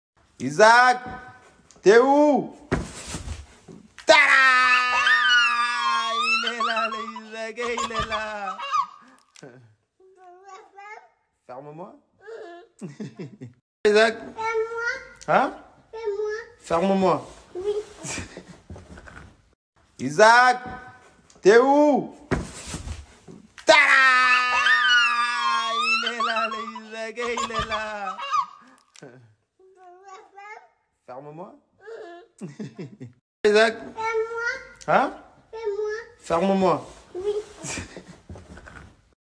Idrissa Gana Guéye enferme son fils Isaac dans une caisse Adidas😂😅. À mourir de rire 🤣😂😀
Idrissa Gana Guéye et son fils Isaac